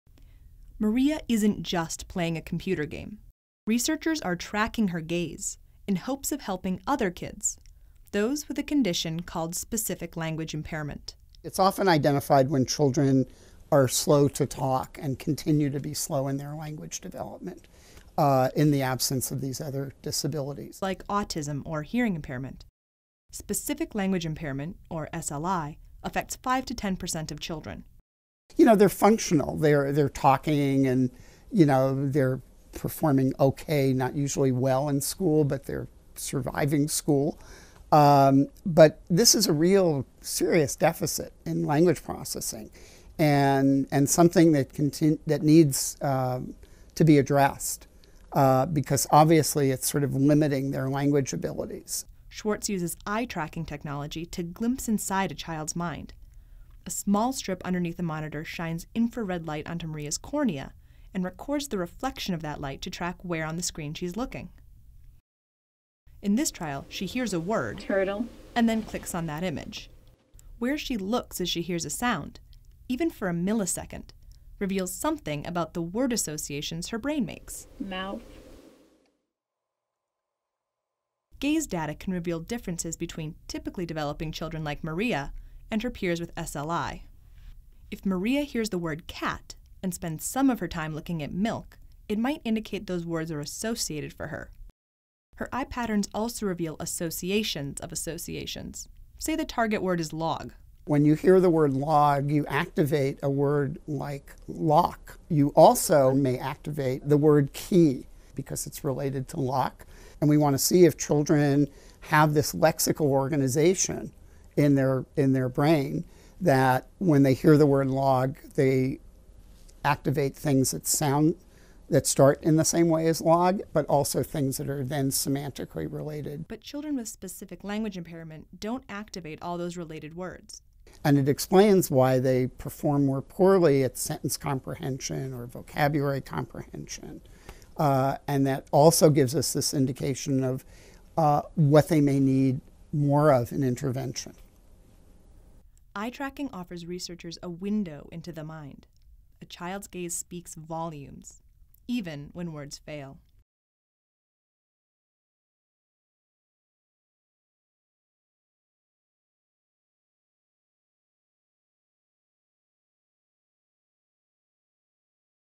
Tracing a Gaze to Understand Language Delays
Researchers use eye-tracking software to peek inside a child's mind when words fail, reading eye patterns to understand language production and combat conditions such as specific language impairment.